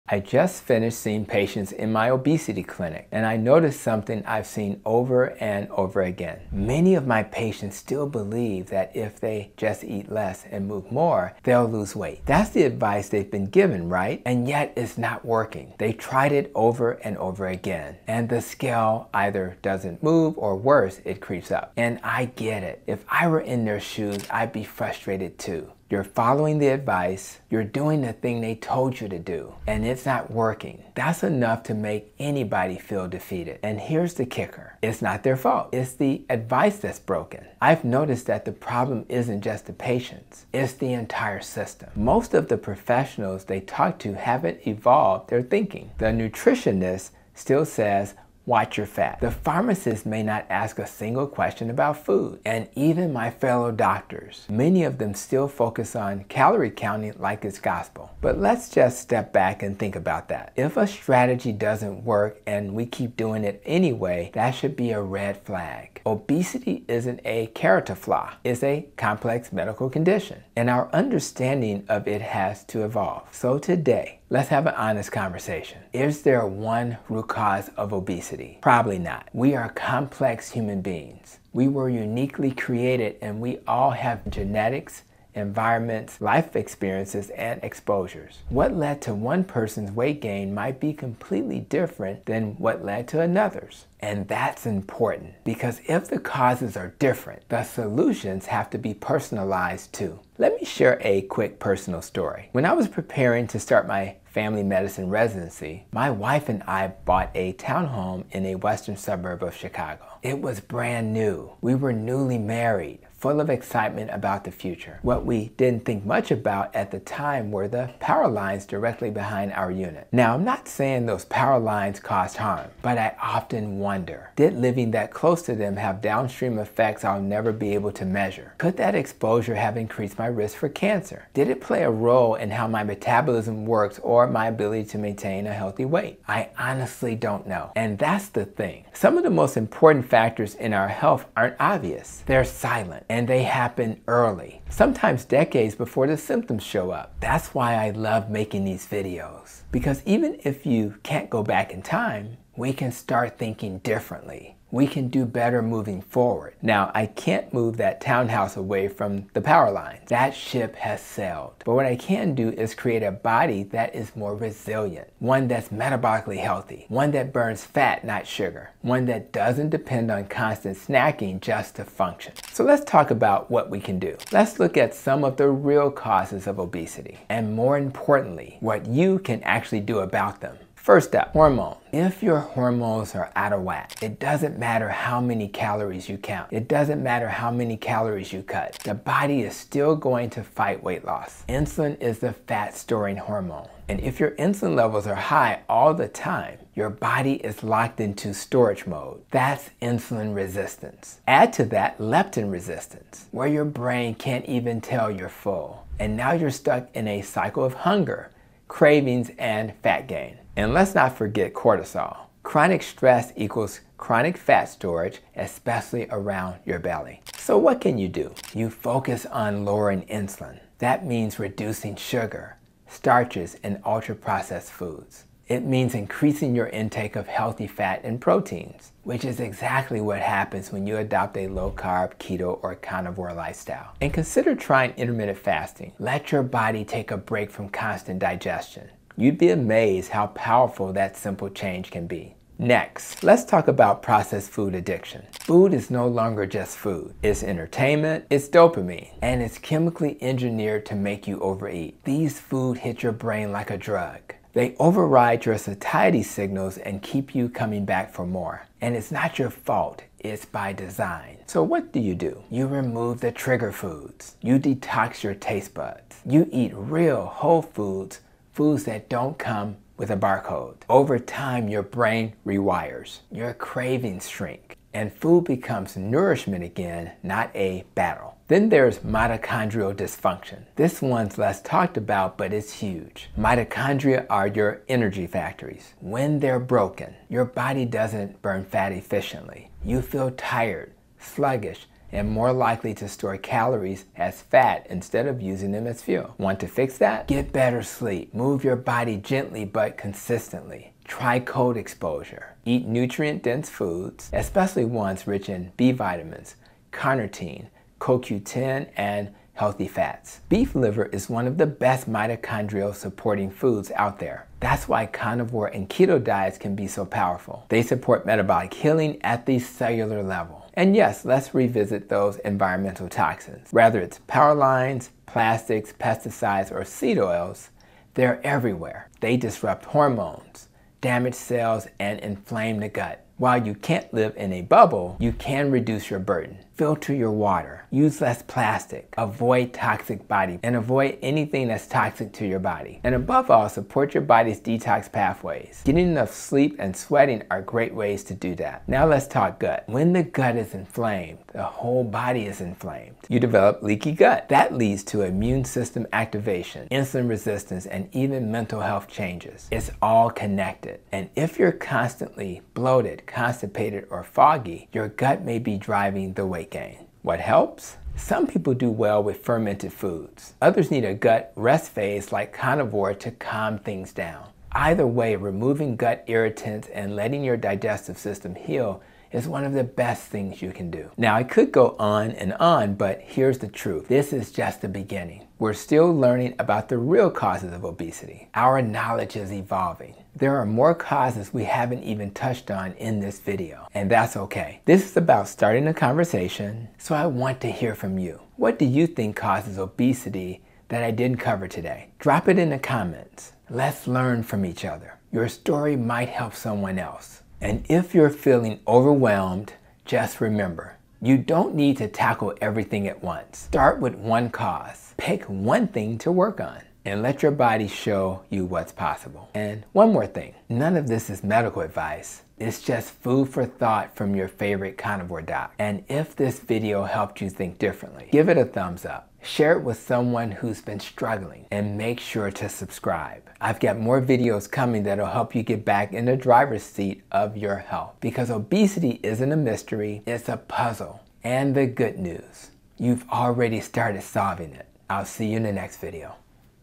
The Real Cause Of Obesity
Most people think obesity is caused by eating too much and not exercising enough—but what if that’s only a small piece of the puzzle? In this video, Dr. Tony Hampton, a board-certified obesity and family medicine doctor, reveals the real causes of weight gain: hormonal imbalances, processed food addiction, mitochondrial dysfunction, gut issues, and even environmental exposures you may not be aware of.

You’ll not only learn the science behind fat storage and metabolic dysfunction—but more importantly, what you can do about it. Whether you're struggling to lose weight or just want to understand your body better, this video will give you a new lens through which to view obesity, healing, and long-term transformation.

✅ Watch until the end to discover Dr. Hampton’s practical tips for reversing these hidden causes—and join the movement to reclaim your health with truth, not trends.

⏱ Video Timeline (One-Liner Per Topic)
00:00 Why “eat less, move more” fails so many people
00:32 Patient frustration: following the rules, but gaining weight
01:15 Obesity is not caused by one thing—it's complex
01:40 Personal story: power lines and environmental exposure
03:00 Hormonal dysfunction explained: insulin, leptin, cortisol
04:02 Processed food addiction: it’s real, and it's engineered
04:38 Mitochondrial dysfunction: the energy crisis inside your cells
05:20 Environmental toxins and endocrine disruption
05:48 Gut health and inflammation’s role in obesity